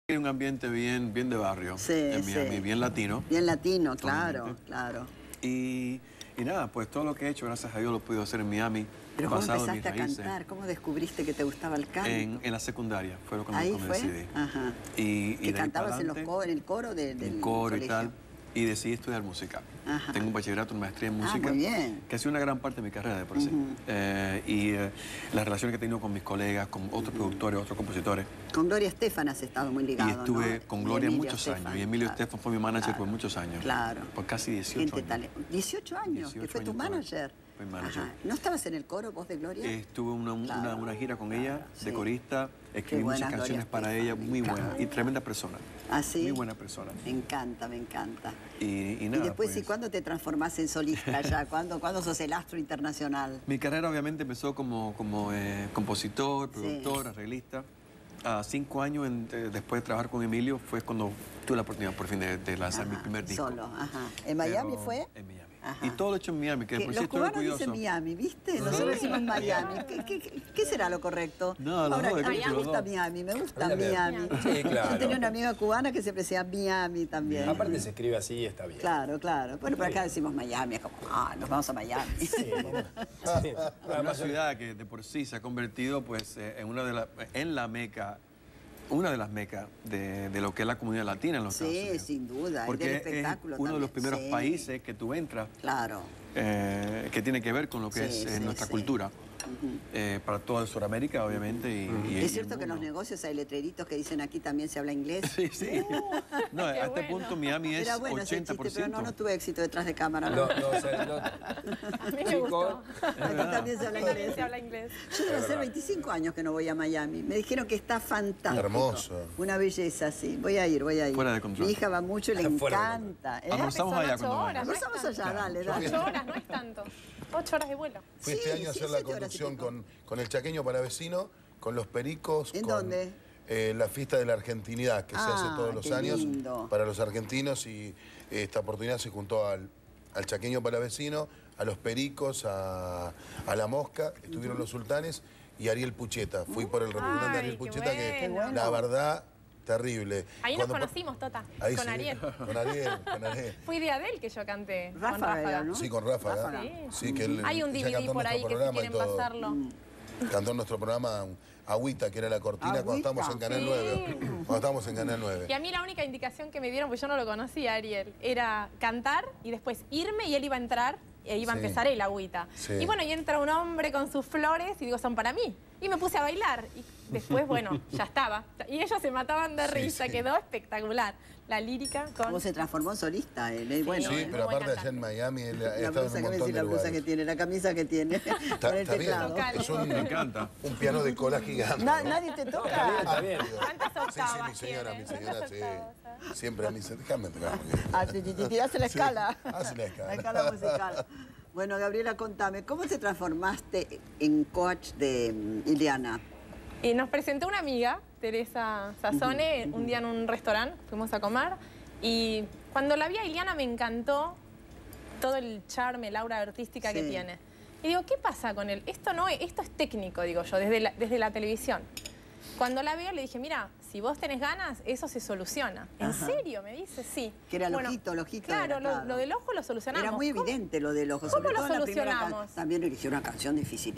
MIRTA LEGRAND JON SECADA MONCHI BALESTRA
estuvimos invitados y comparti la mesa junto a mi amigo Jon Secada, canal América tv, bsas Argentina